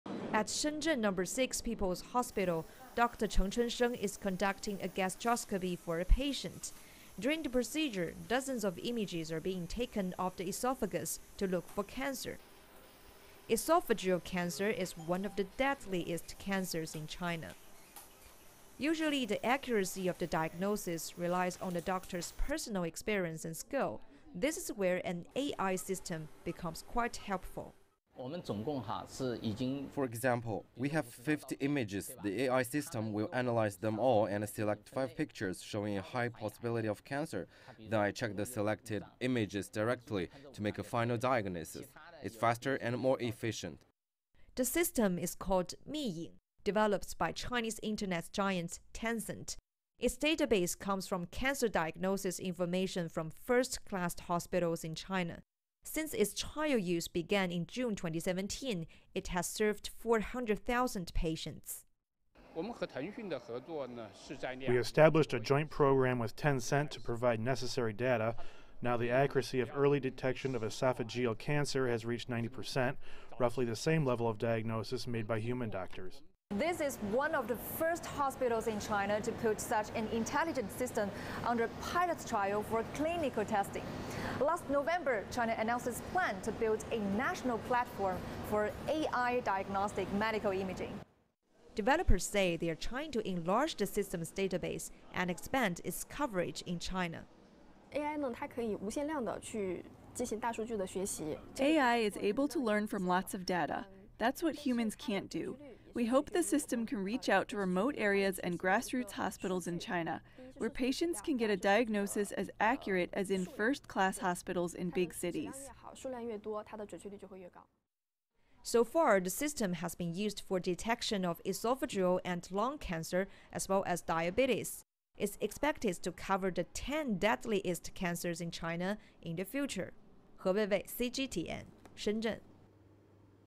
Chinese hospitals use AI technology to detect cancer
The Boao Forum is not just about economies, but also innovation -- and the chance to change lives. China is exploring artificial intelligence applications in various areas. It's recently made a breakthrough in the medical sector. A-I technology is being used to detect cancer. CGTN's He Weiwei has more.